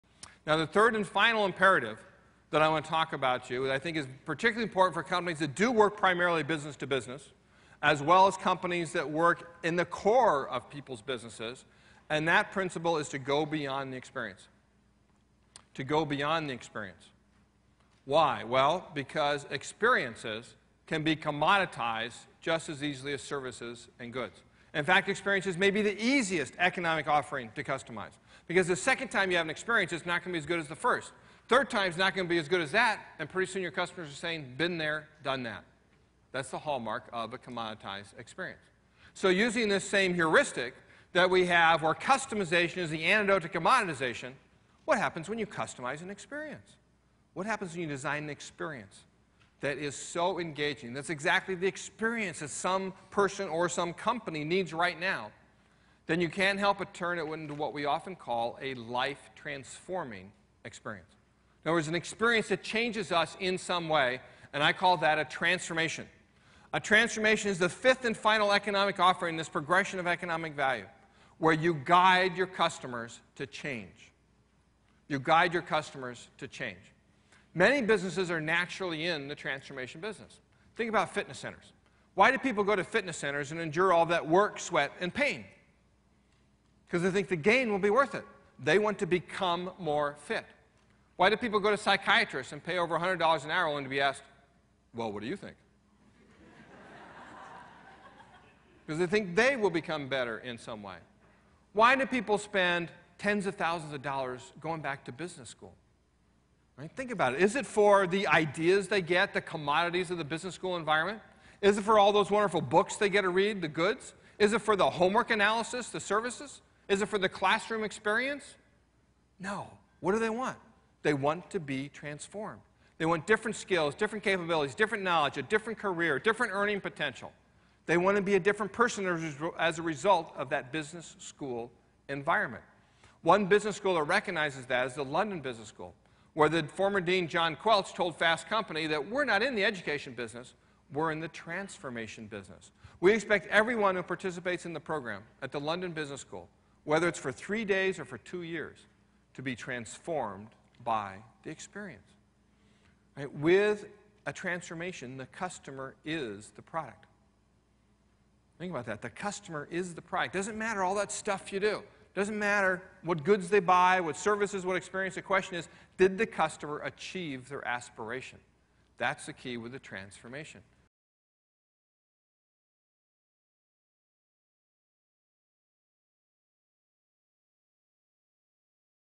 Going Beyond the Experience
Joe Pine shows that once Experiences are customized, transformations result where the customer is the product.